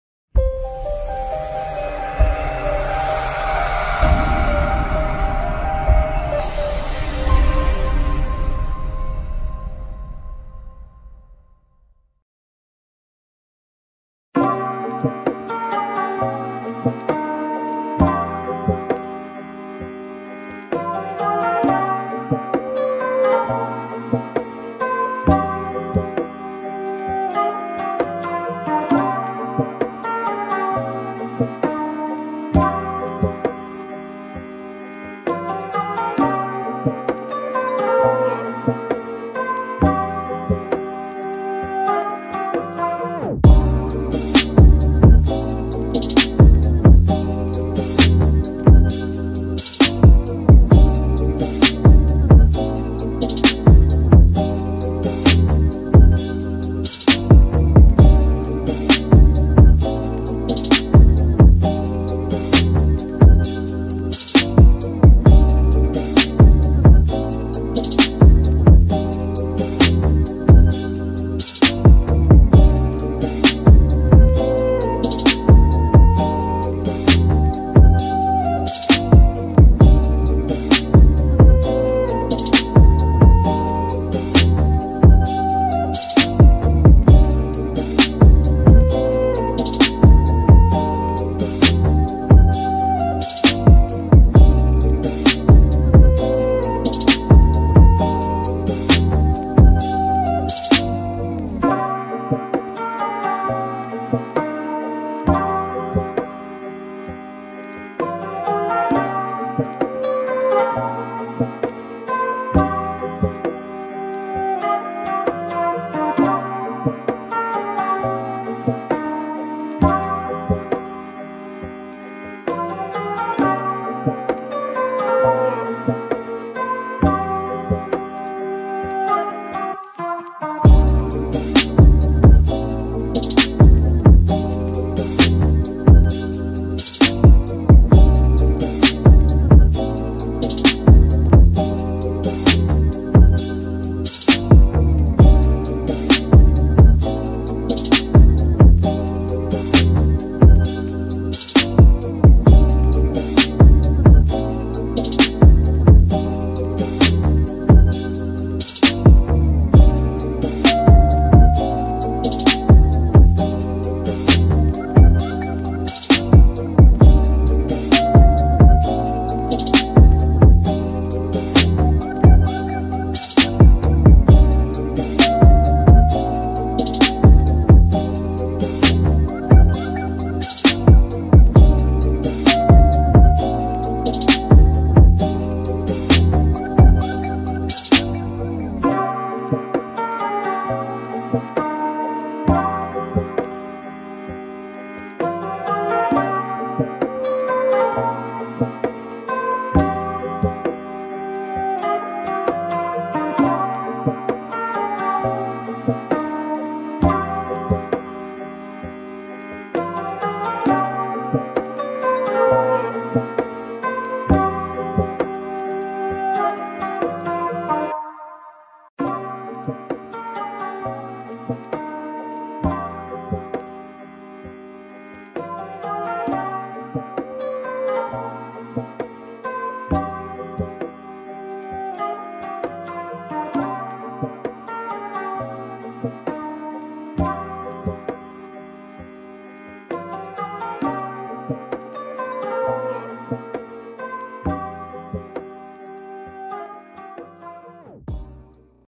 2021 Lofi hip hop - Beats to download without copyright
Tienes un poco más de 3 minutos de buen lofi para descargarlo y usarlos para tus vídeoclases como sonido de fondo.